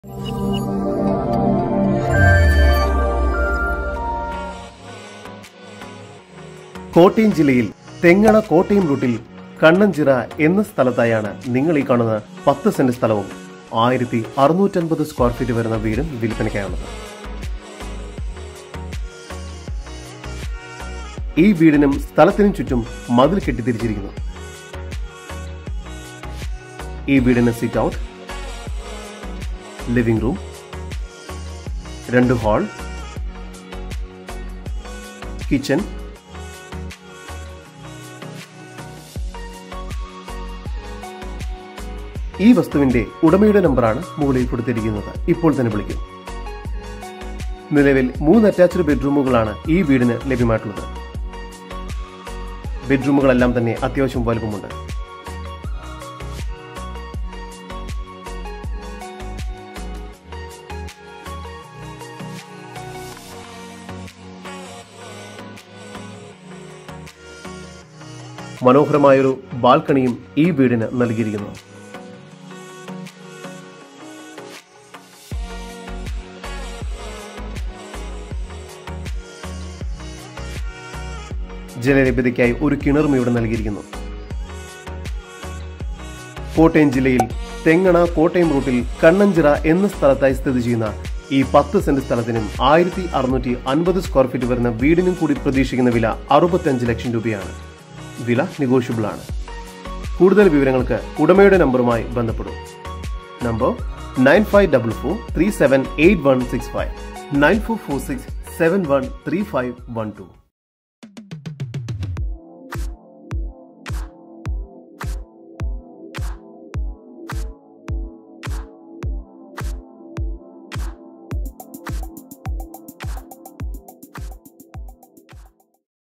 House Sale In Kottayam Thengana | Contact Owner
House Sale In Kottayam
10 Cent 1650 Sqft
3 Bhk
Asking Price 65 Lakhs Negotiable
Owner  Name  : Mrs. Josamma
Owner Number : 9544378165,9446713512

നിങ്ങളുടെ പ്രോപ്പർട്ടീസ് ഇങ്ങനെ വീഡിയോ പരസ്യം ചെയ്ത് വിൽക്കുവാൻ വിളിക്കുക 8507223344

Published by

JUS MEDIAS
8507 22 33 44